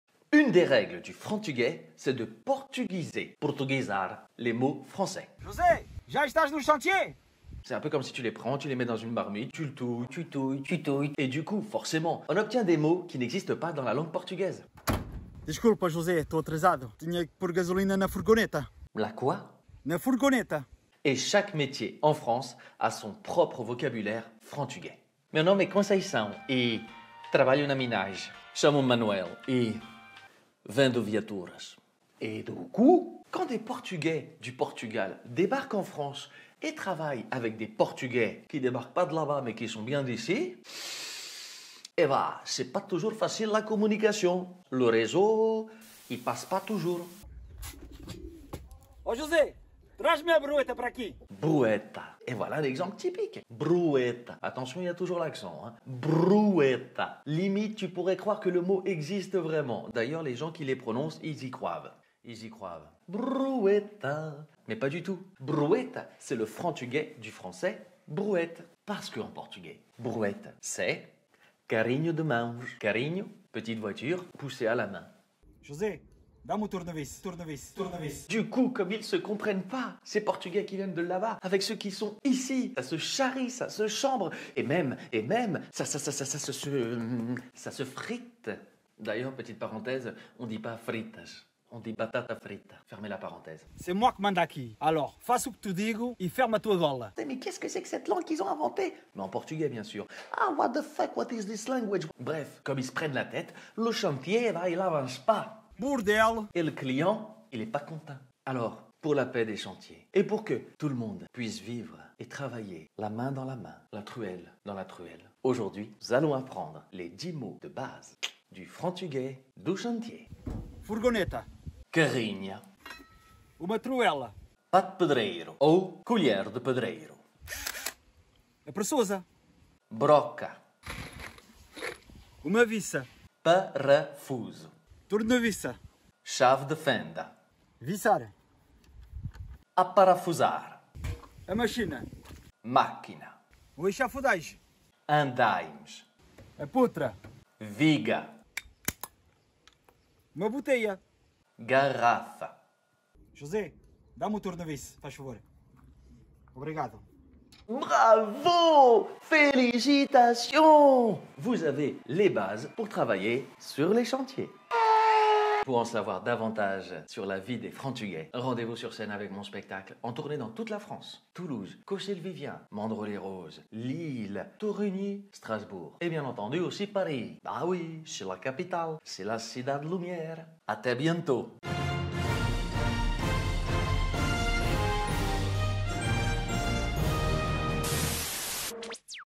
LE FRANTUGAIS - O FRANTUGUÊS - DO CHANTIER 5/9
Le 1er tuto pour réussir son intégration sur les chantiers

Idéal pour les Frantugais et Frantugueses
langues parlées : Français, Portugais, Frantugais, Frantuguês

avec la participation de David Moka
obrigado meu amigo